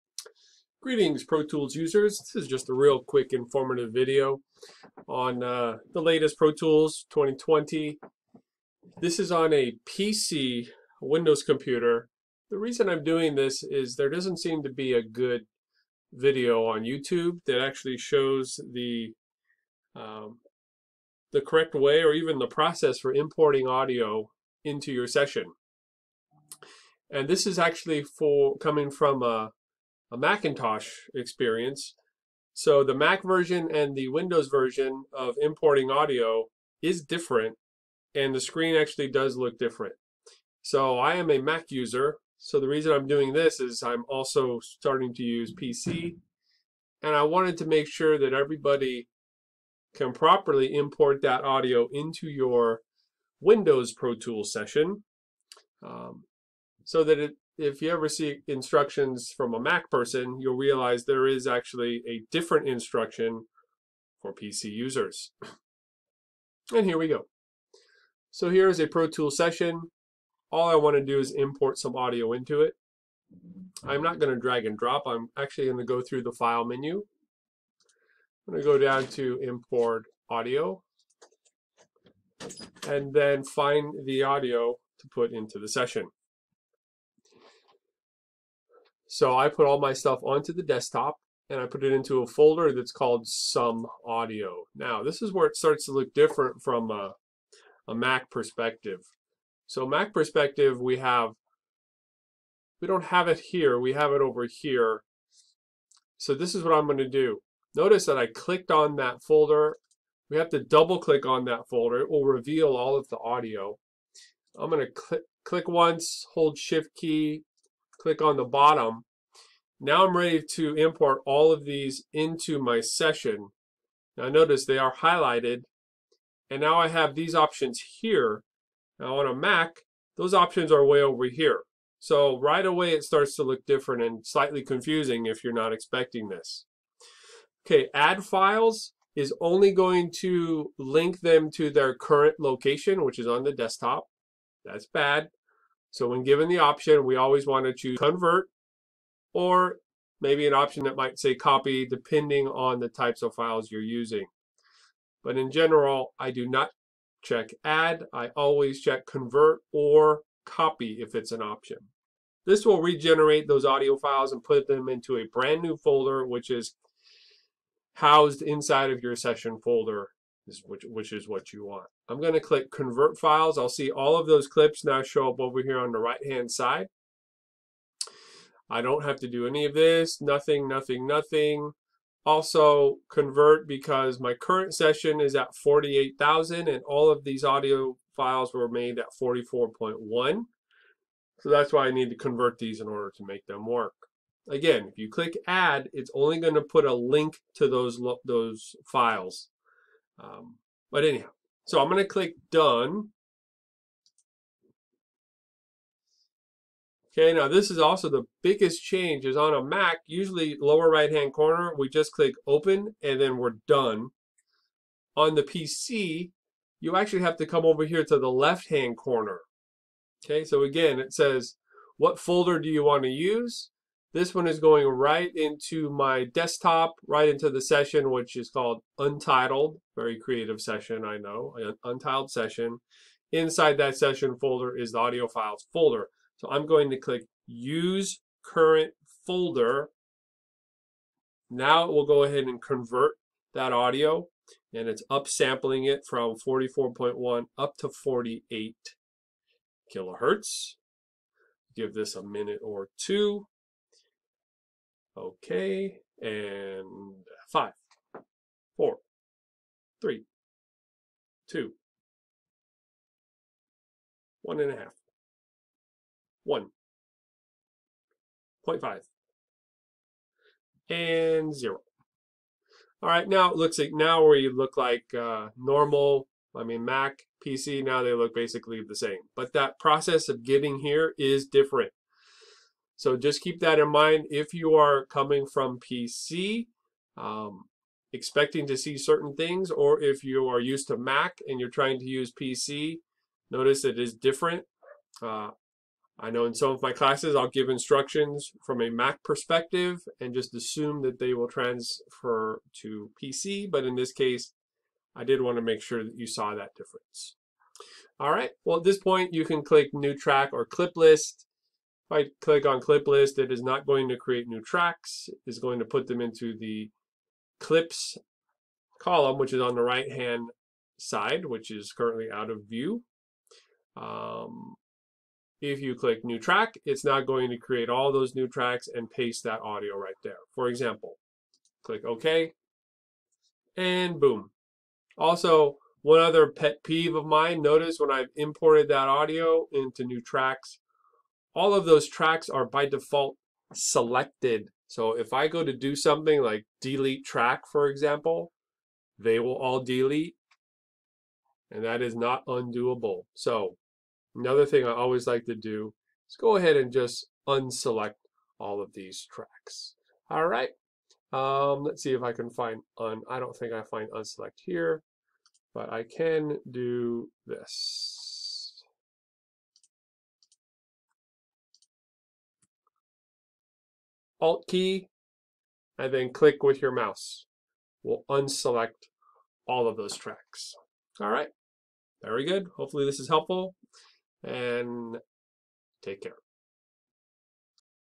Importing Audio In Pro Tools on a PC
Quick instructions on using File menu to import audio into Pro Tools using a PC